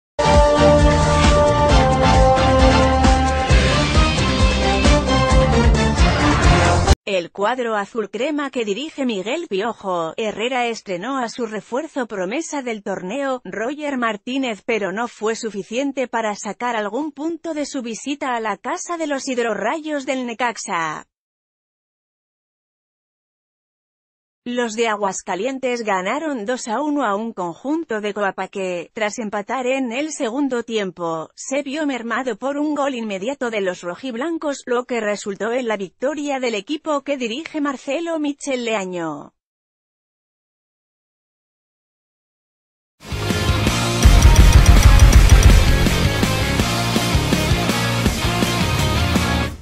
América fue sorprendido por un Necaxa que le arruinó su debut en el Apertura 2018
El cuadro azulcrema que dirige Miguel "Piojo" Herrera estrenó a su refuerzo promesa del torneo, Roger Martínez, pero no fue suficiente para sacar algún punto de su visita a la casa de los Hidrorayos del Necaxa. Los de Aguascalientes ganaron 2-1 a un conjunto de Coapa que, tras empatar en el segundo ...